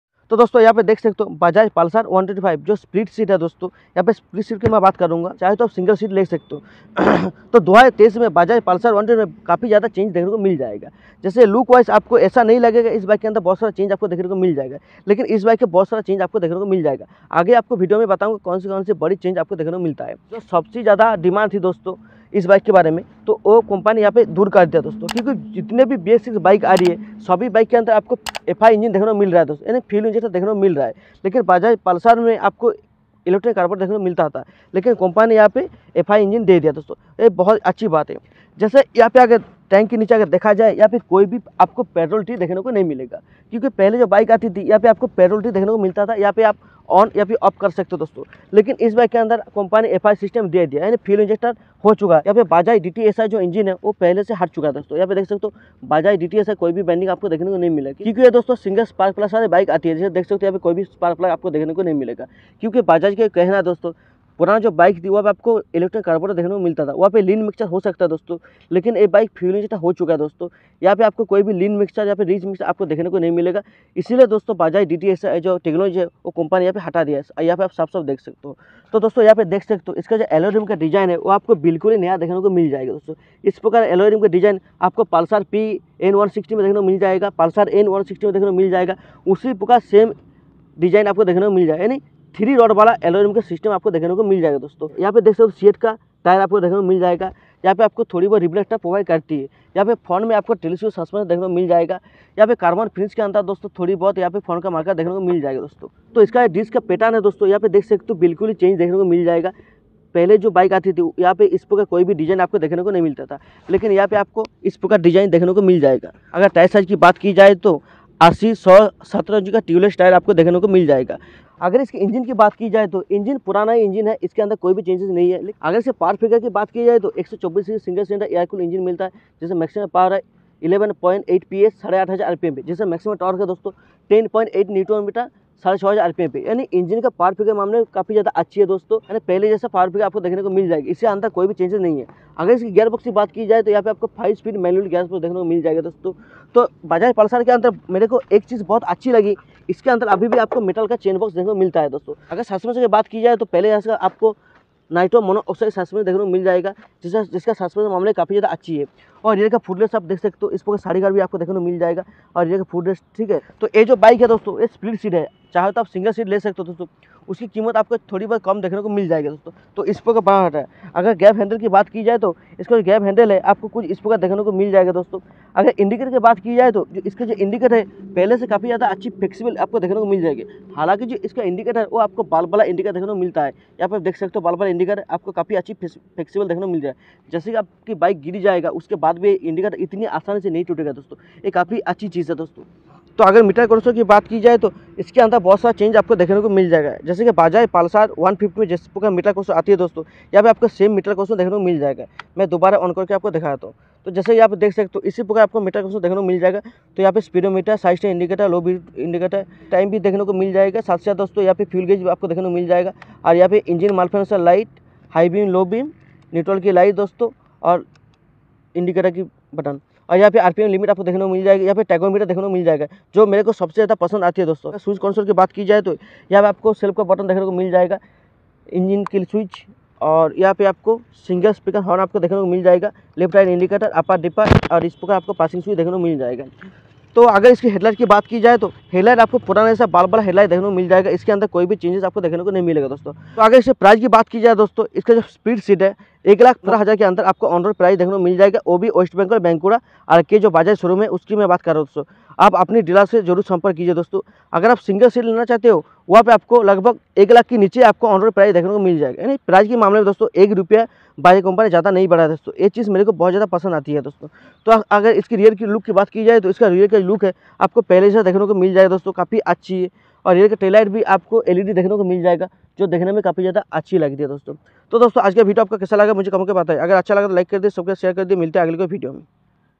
New Update Bajaj Pulsar 125cc 2023 Model | Mileage price Details Review Bike Updater
Bajaj Pulsar 125cc
Power 11.8ps@8500rpm
Tork- 10.8nm@6500rpm